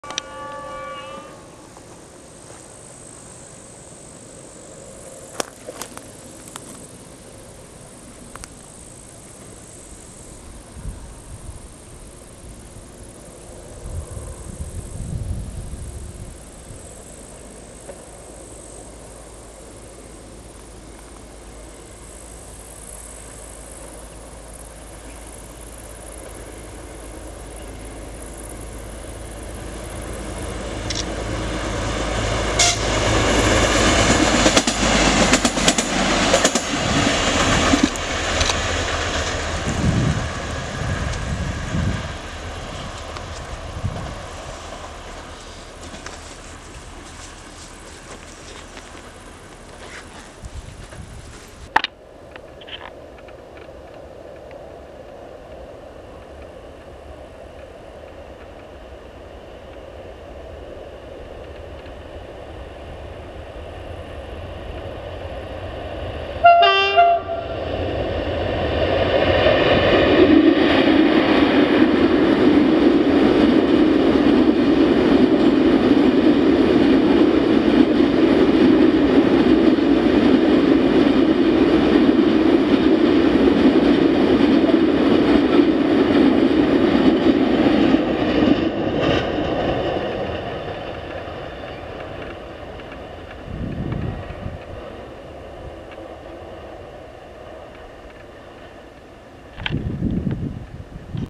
Szynobus na LHS & ET22 z cysternami na prostej pod Kozłowem
Składy jechały z niecałym 2 minutowym odstępem szkoda bo by wyszła piękna jazda równoległa